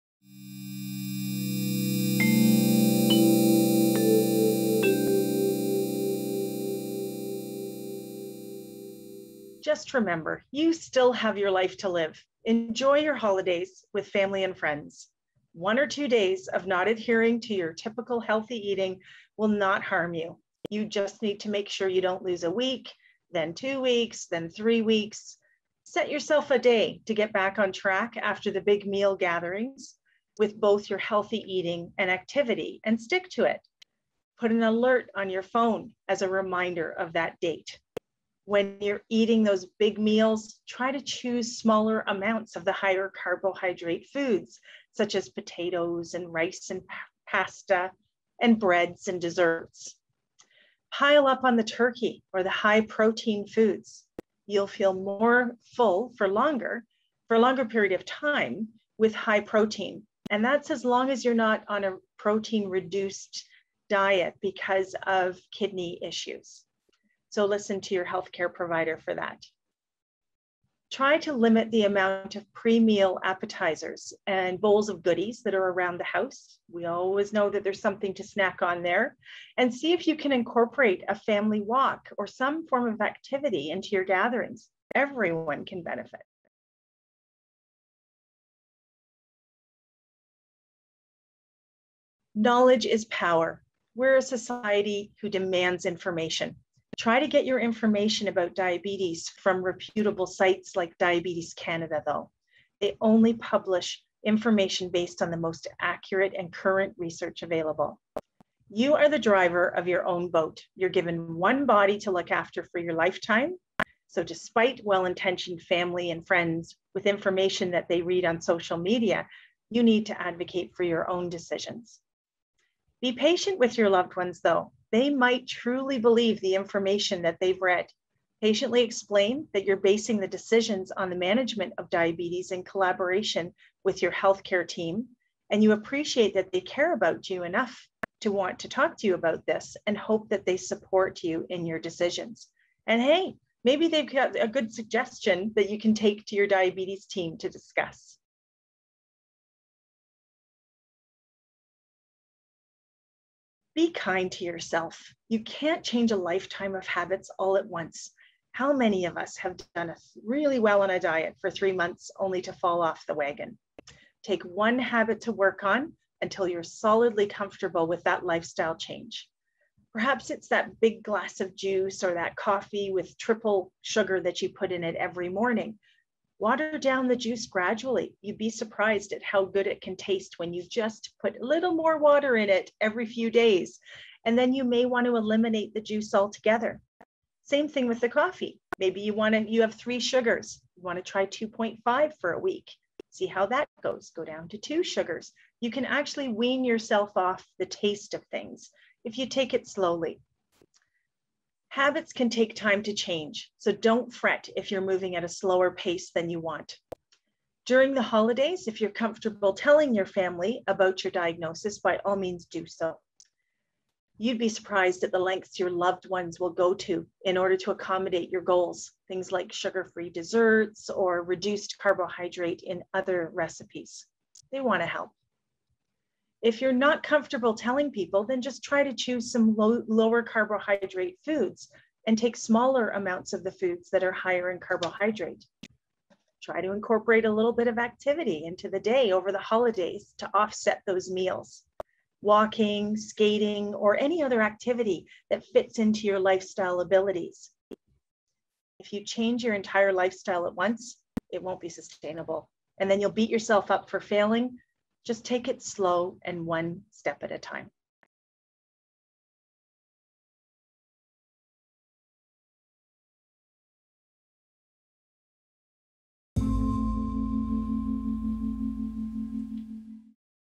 Celebrating with Diabetes | Ask the Experts from Diabetes Canada
Check out our last episode of Ask the Experts to find out answers to your diabetes questions. In this episode, learn more about celebrating, tips for self-management, and advice for navigating life with diabetes during the holiday season.

Questions, answered by Lisa Maks, RN, MN, CDE | Clinical Nurse Specialist, Diabetes:
- I live with diabetes and I'm nervous about the food & drink traditions associated with the holidays. What strategies can I use? (0:05)
- How can I advocate for my health needs with family & friends? (1:26)
- I was recently diagnosed with diabetes. What do I need to know about the holidays? (2:28)